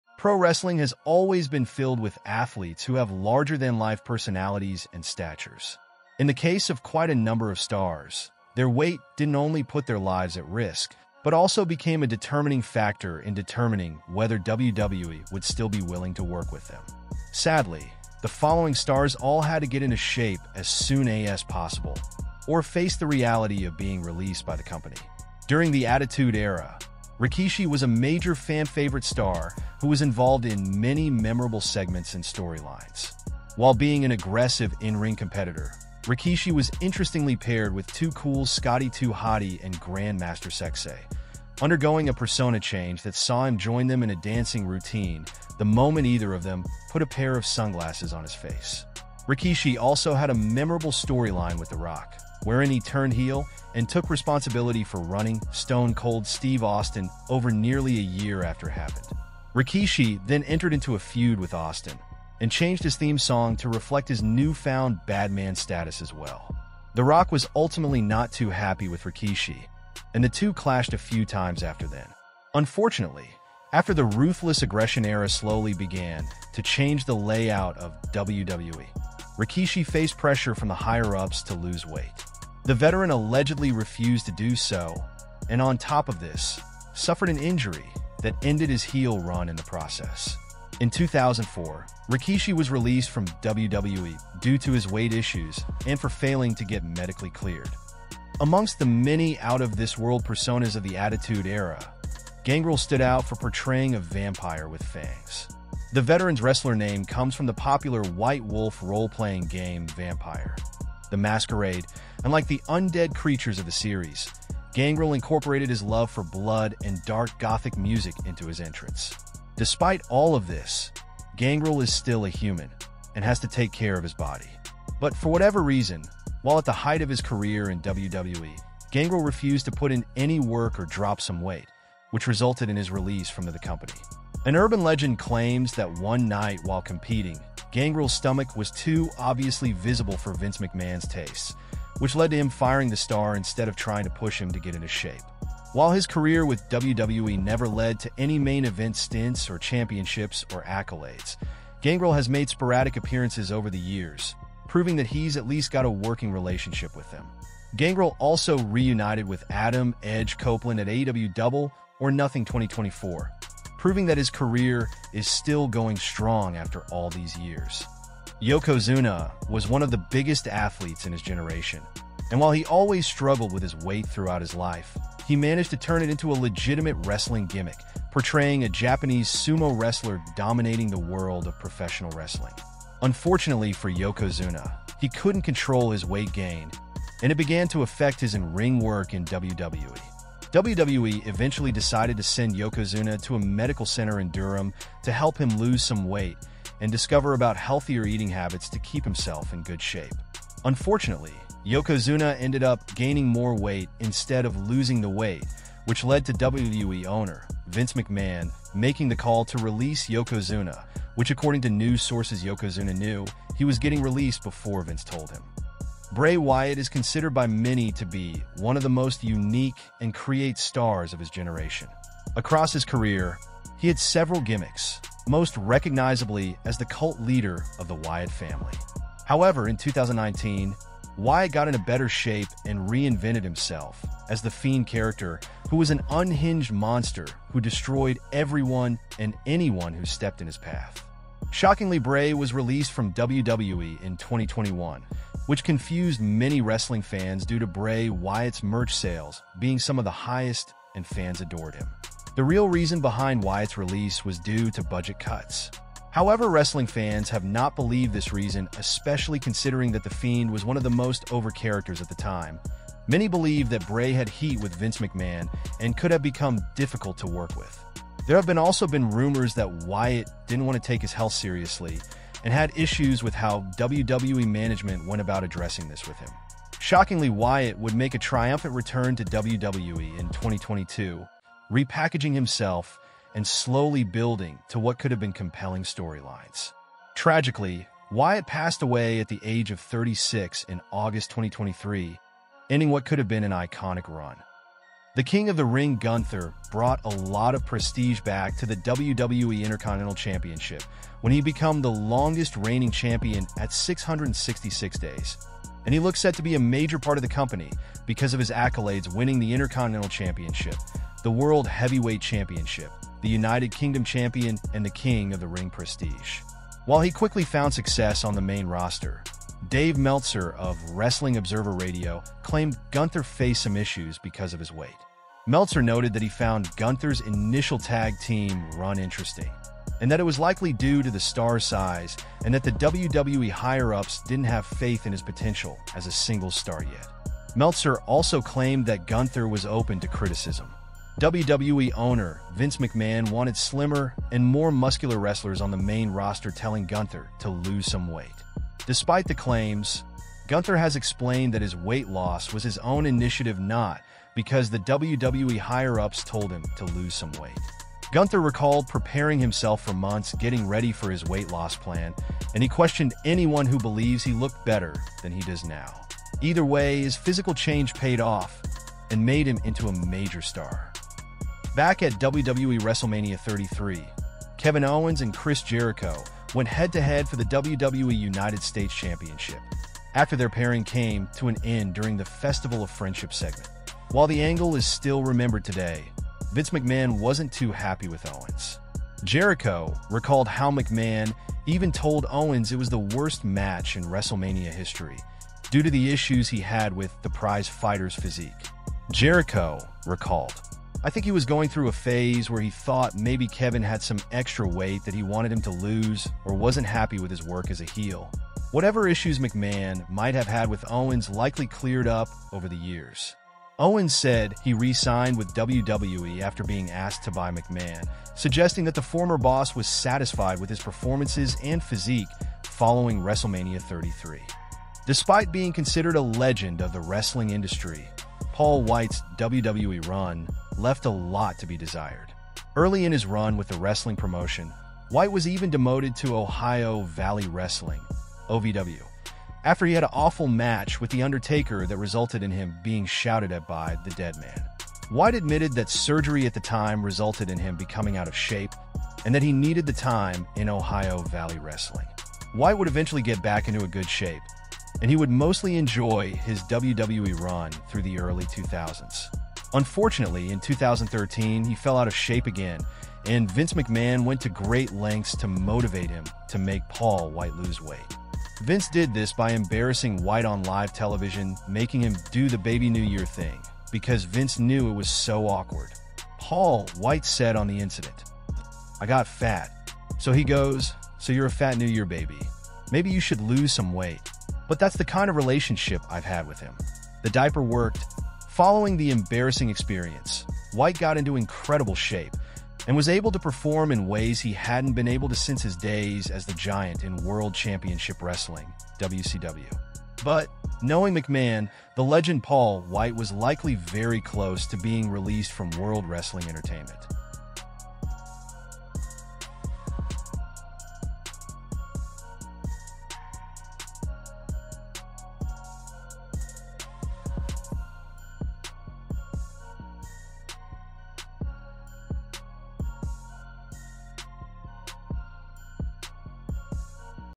Wrestlers Who Were Told To Lose Weight Or They Would Be Fired!
This is a video on WWE Wrestlers Who Faced Weight Loss Ultimatums to Keep Their Jobs!